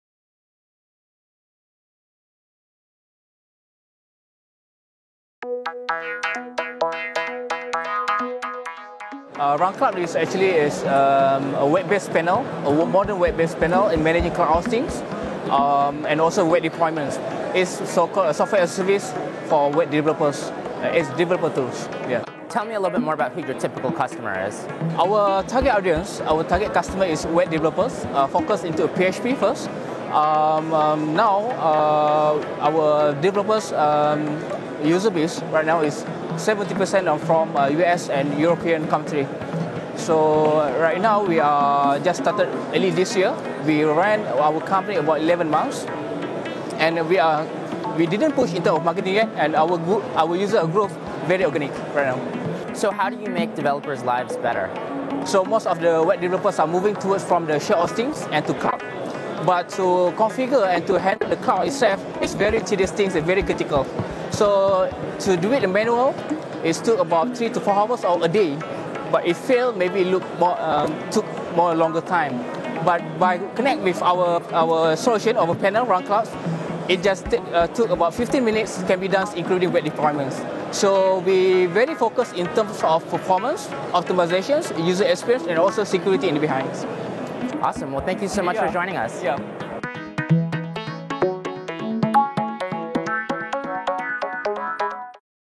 VidAds! : Run Cloud Web Hosting info graphic ads introduce product awareness
In This video i will shows you some product ads way to Create your WordPress Website with Free domain name and Free hosting. This video for those who want to start their small business or initially they just want to experiment with website and want to gain their knowledge about how WordPress website works ? I will try to do my best to show you every single step.